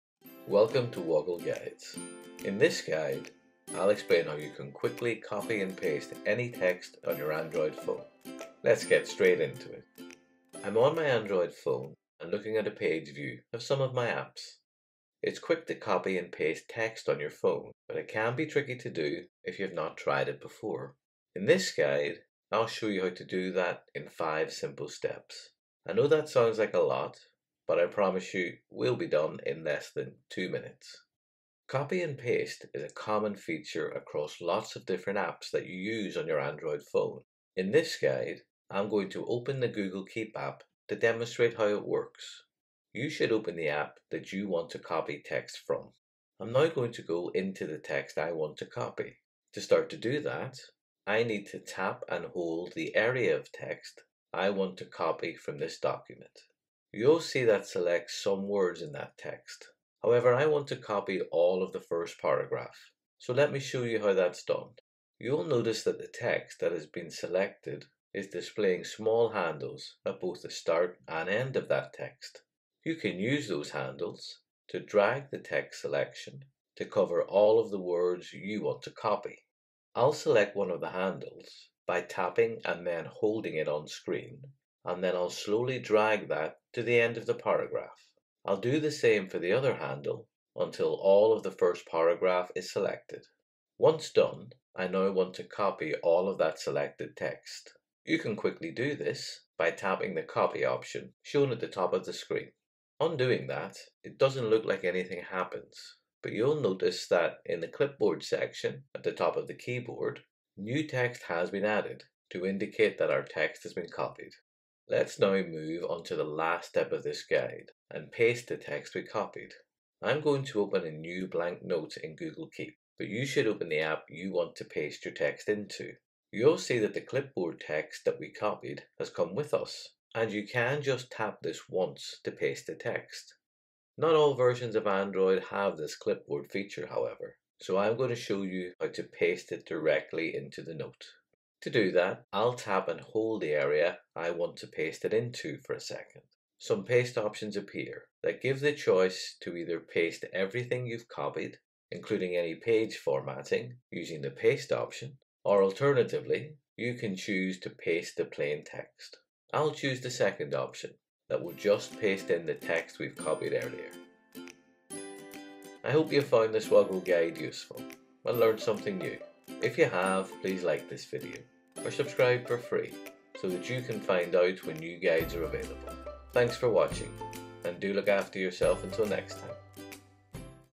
How to copy and paste on Android
In this guide, I’ll explain how you can quickly copy and paste any text on your Android phone.

*🕔  Key Moments*
00:00 | Introduction
00:09 | How to copy and paste on your Android phone
02:45 | Wrap up